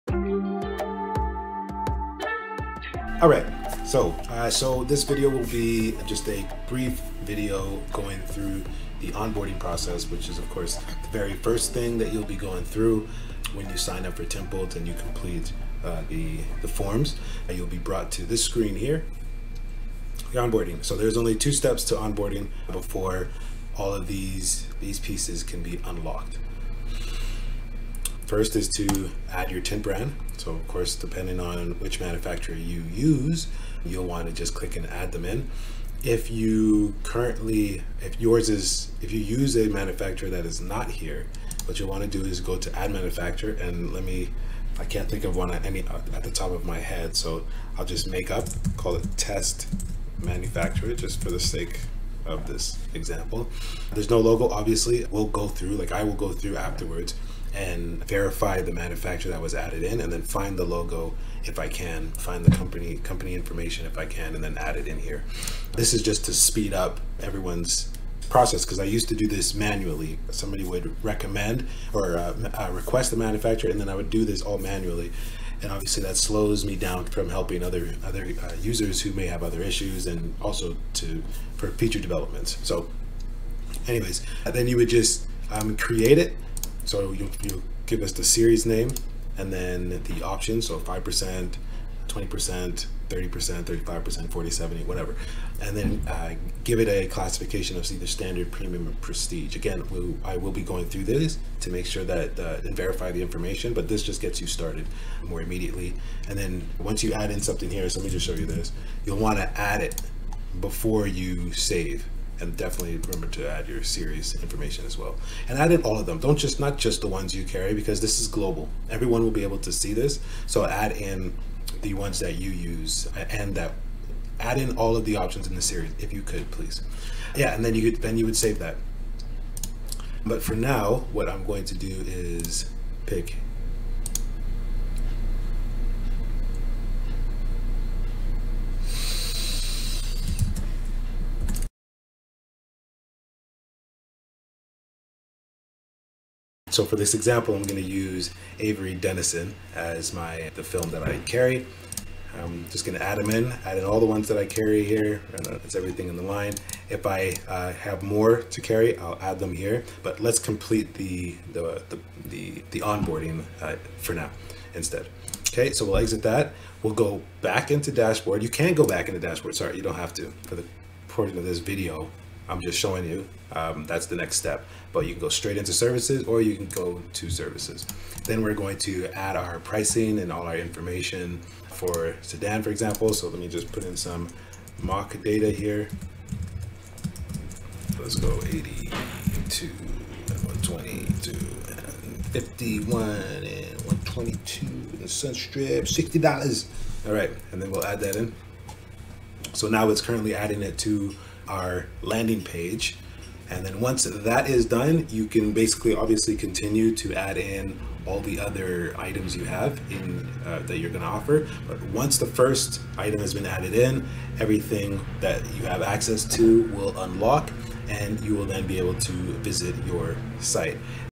Tint Bolt Steps To Success - Step 1 - Setup Tint Bolt (Onboarding walkthrough video)
This video is part of the internal Tint Bolt walkthrough videos and is also the first step you take to get the most out of Tint Bolt.

Tint Bolt is a automation and marketing tool for automotive window tinters that want a better solution to the standard simplistic website options out there.